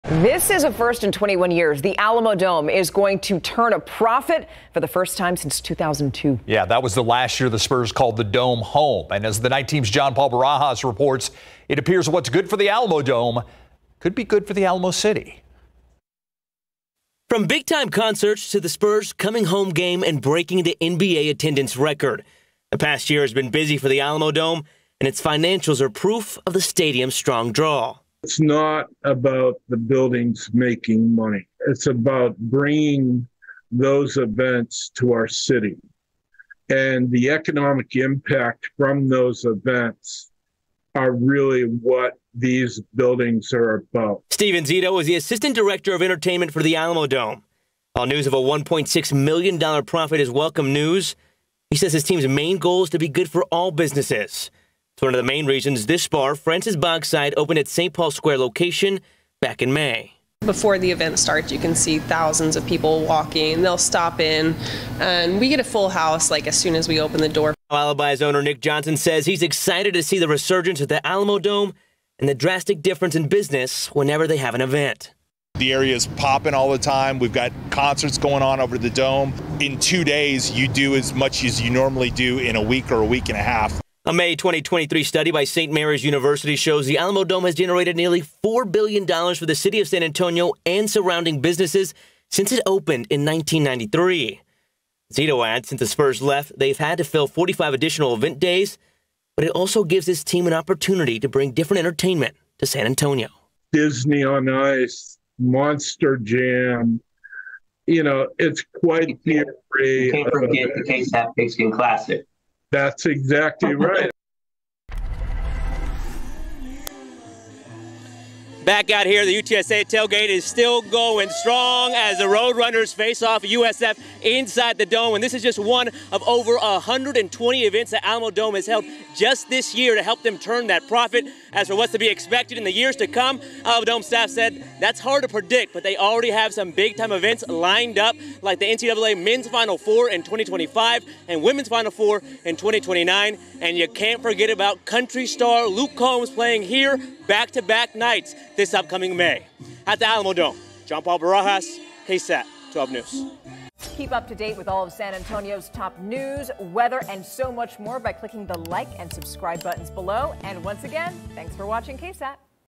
Alamodome expected to turn a profit for the first time in 21 years, officials say
For the first time in 21 years, the Alamodome says it will turn a profit.